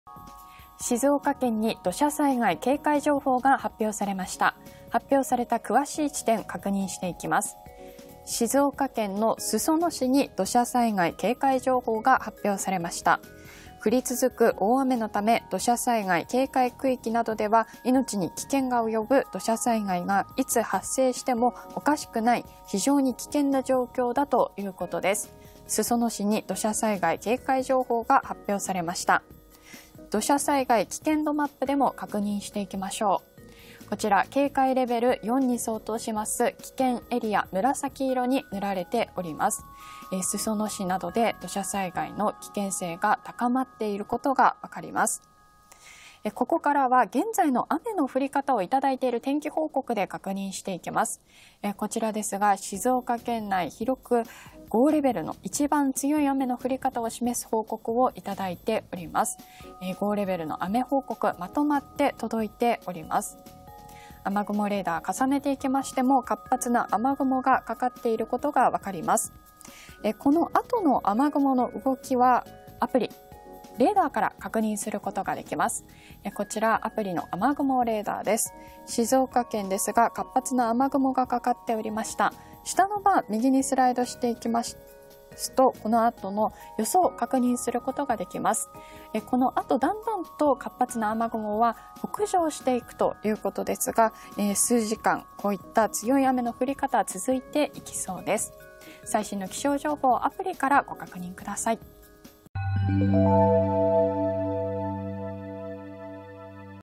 静岡県に土砂災害警戒情報発表
静岡県土砂災害警戒情報を発表　降り続く大雨のため、土砂災害警戒区域等では命に危険が及ぶ土砂災害がいつ発生してもおかしくない非常に危険な状況です。

【警戒対象地域】裾野市*

崖の近くや谷の出口など土砂災害警戒区域等にお住まいの方は、市町から発令される避難指示などの情報に留意し、少しでも安全な場所への速やかな避難を心がけてください。